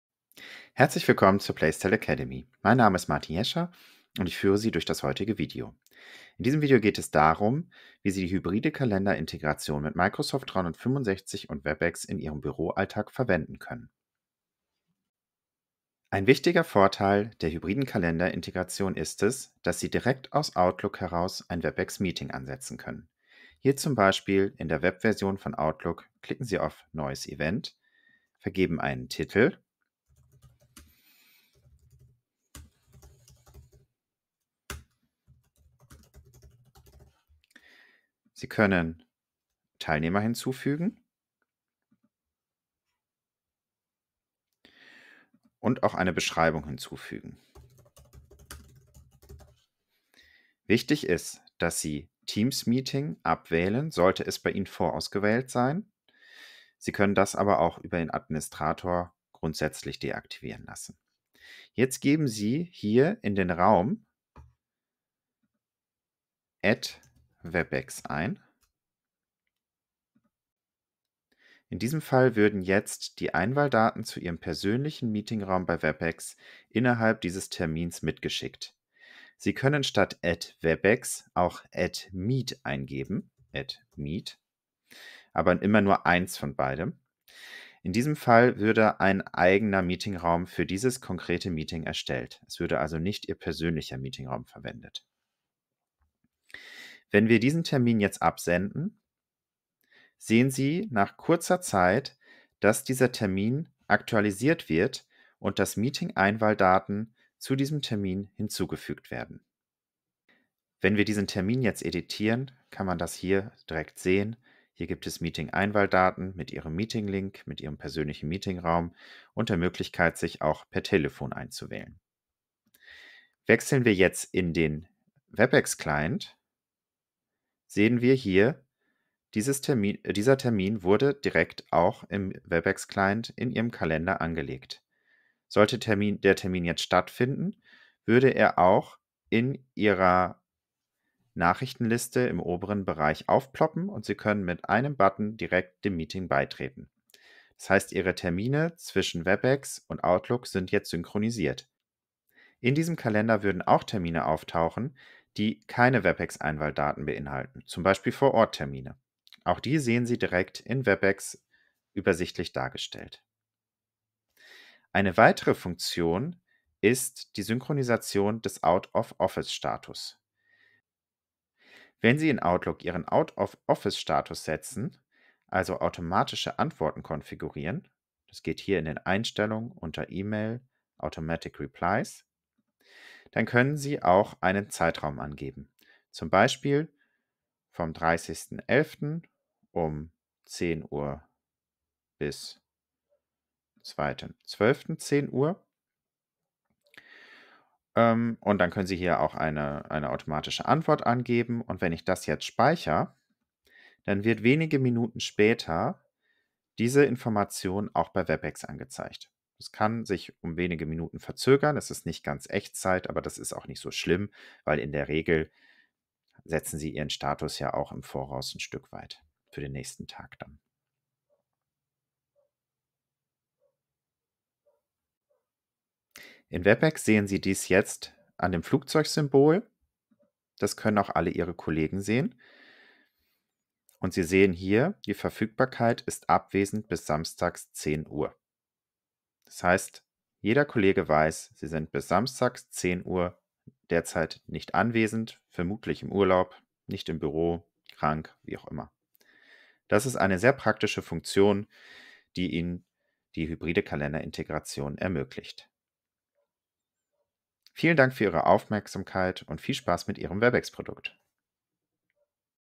Hybride Kalenderintegration mit Microsoft 365 Anwendung bei Webex für Placetel
In diesem Video zeigen wir Ihnen, wie Sie die hybride Kalenderintegration mit Microsoft 365 bei Webex für Placetel anwenden können.